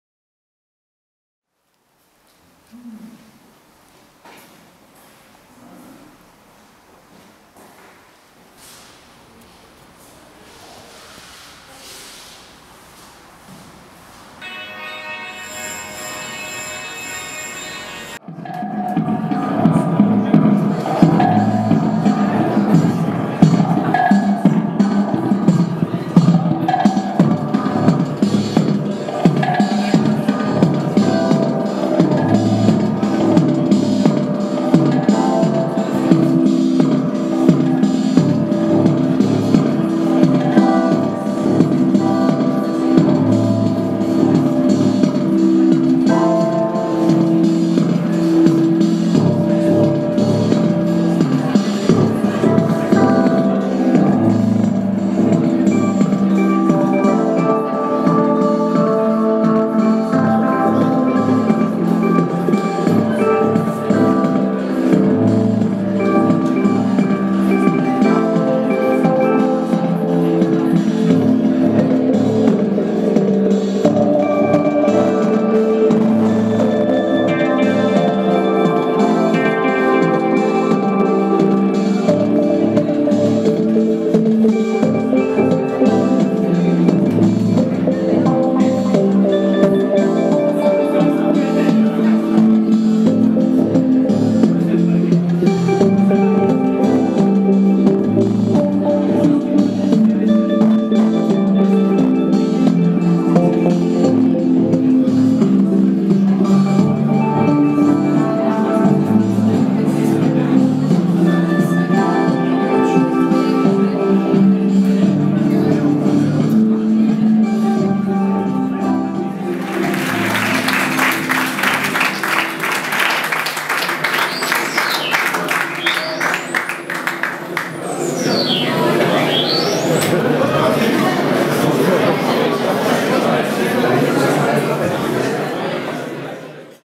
Summer Bummer: Concert by Laurent Baudoux & Roger 3000
SUMMER BUMMER: A residency in the gallery
The Atelier Pica Pica collective residency features collaborations with friends artists:
E.B. Itso, Magnus Vind, Julien Couic, Julien Meert, Laurent Baudoux & Roger 3000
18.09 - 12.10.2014